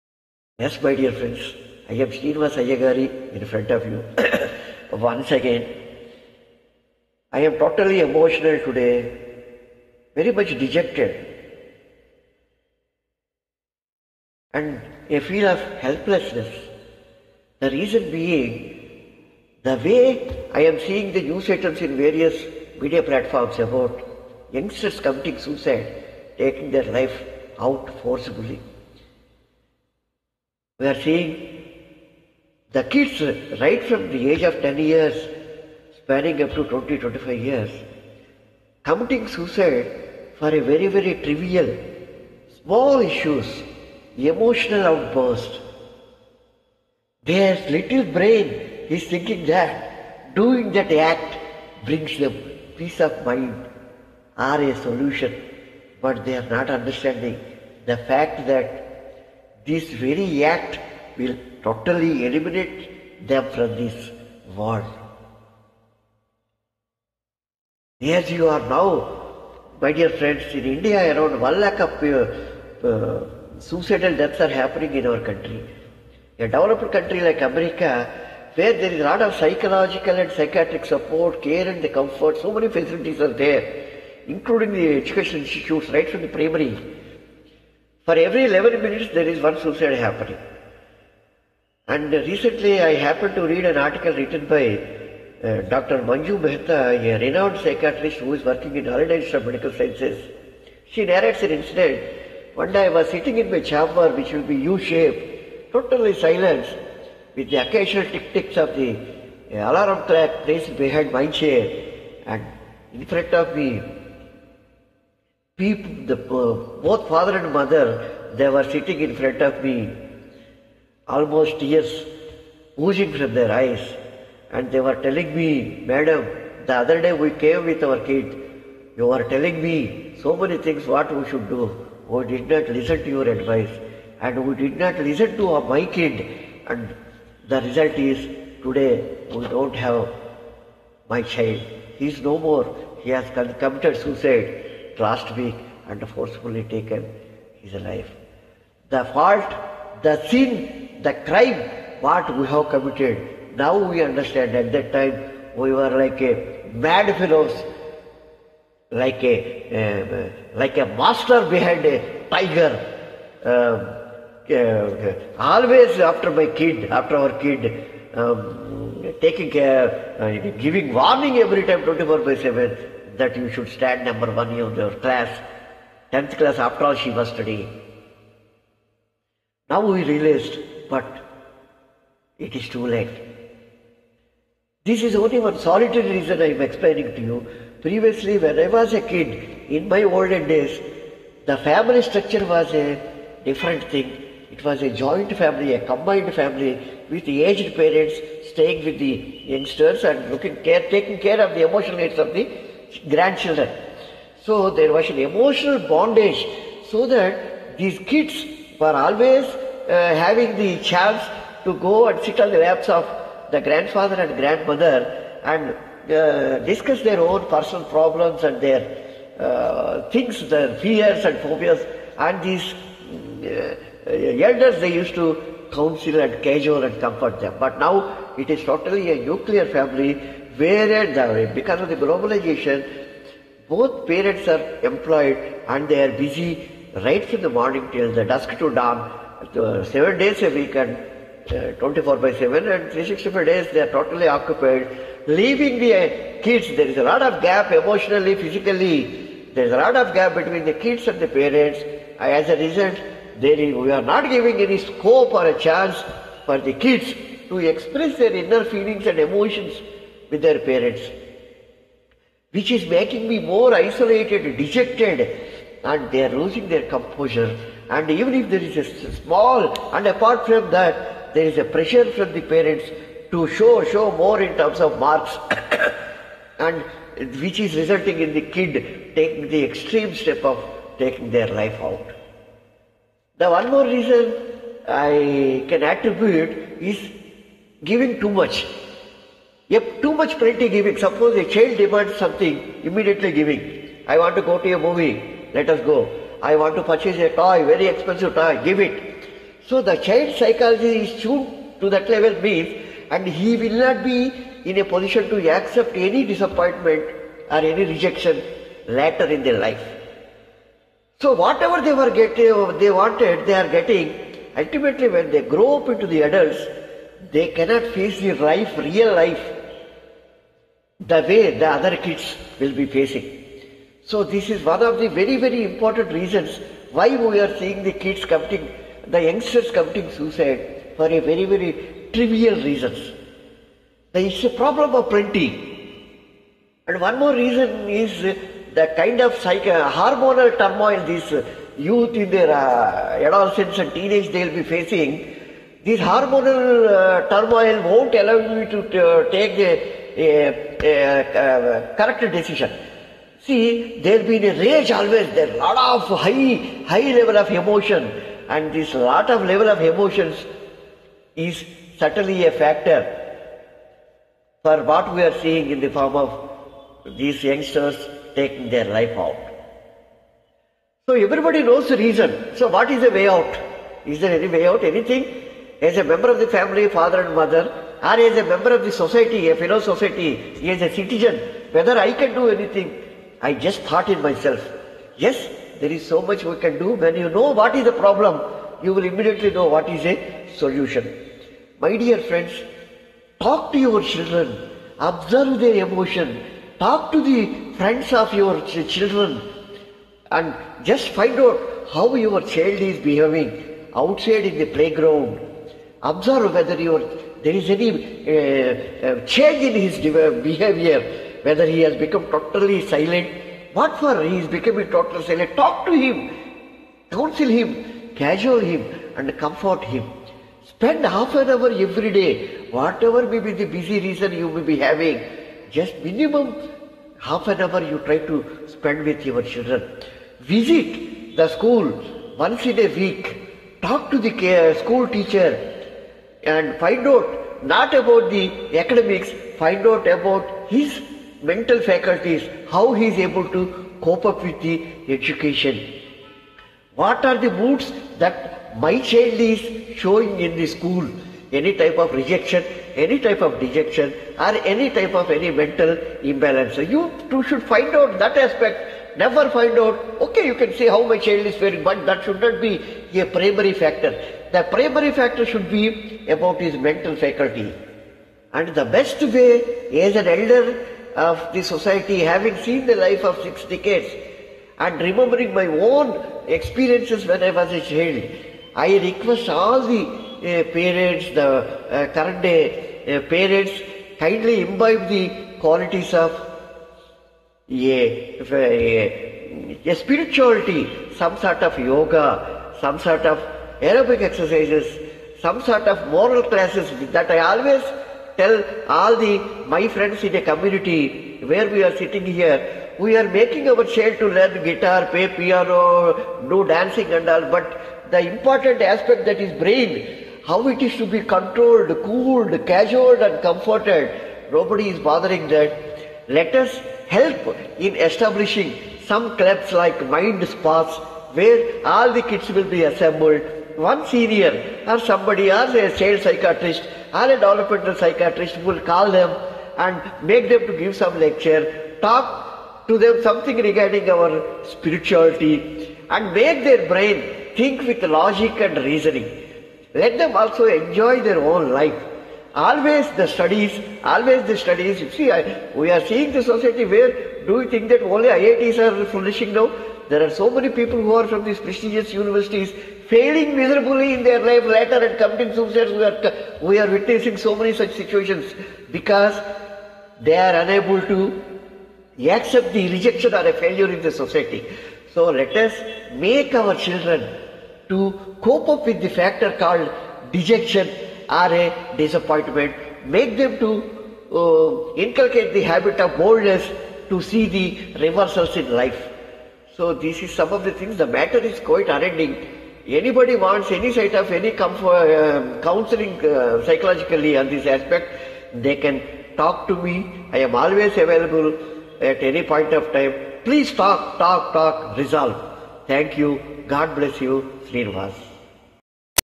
SAY NO TO SUICIDES. TALK BY AYYAGARI SRINIVAS
PLEASE FORWARD THIS VIDEO TO UOUR FRIENDS IN OTHER GROUPS SO THAT THIS IMPORTANT SUBJECT GETS THE MAXIMUM COVERAGE AND BE A HELP TO THEM.